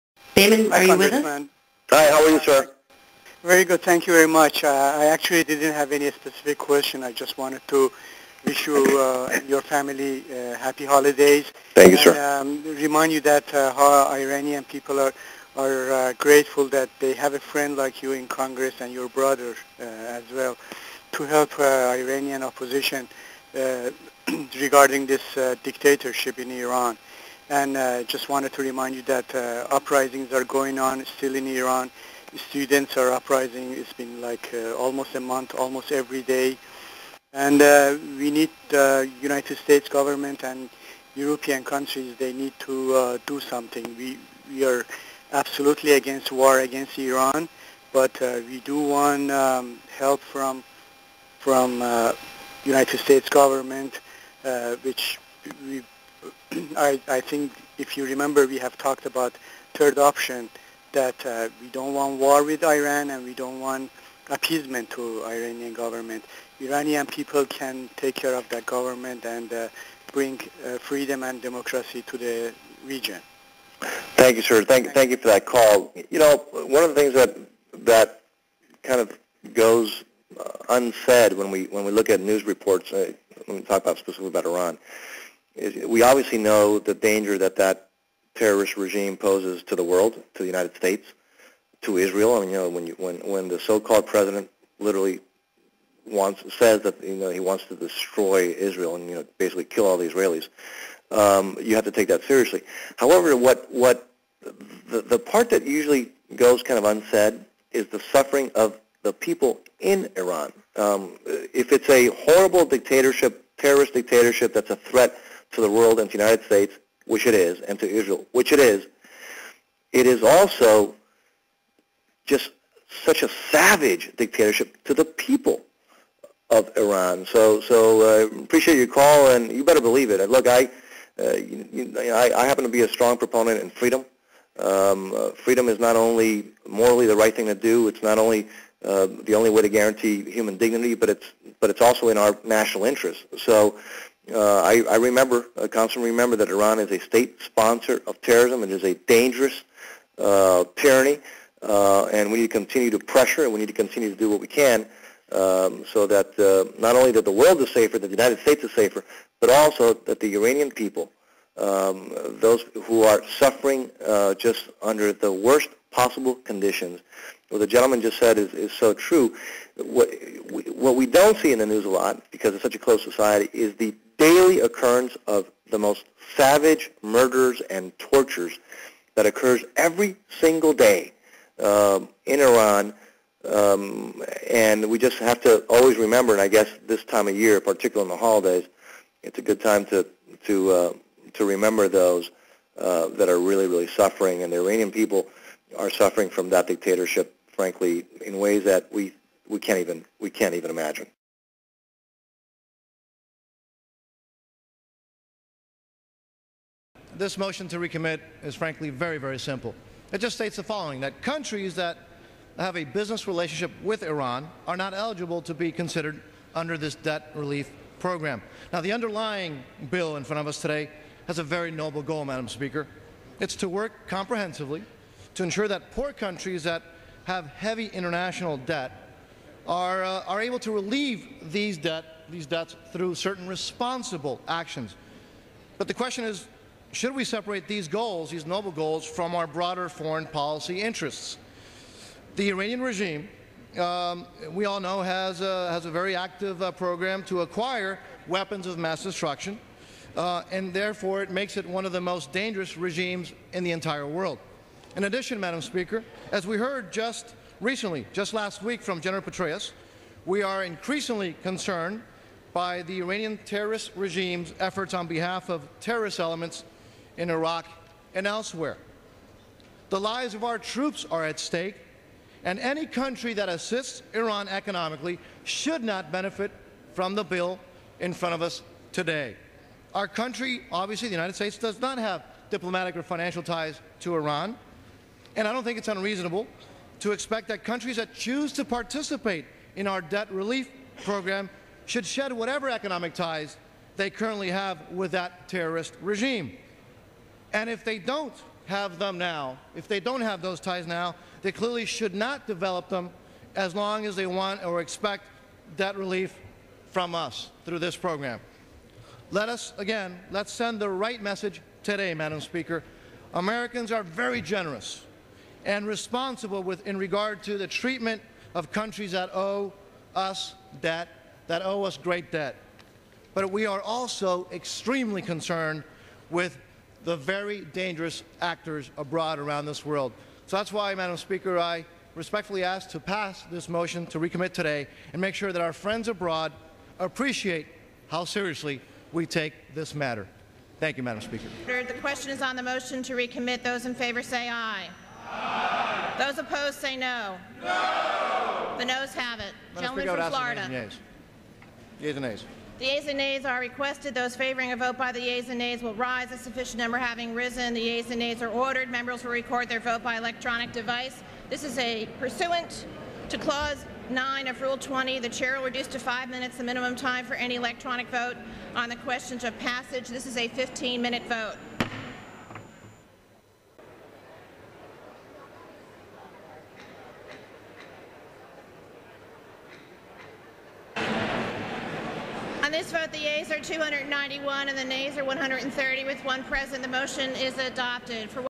Diaz-Balart: District Conference Call 12/18/07 (Part 7 of 9)
Congressman Mario Diaz-Balart hosted a conference call with constituents in his district to hear what was on their minds.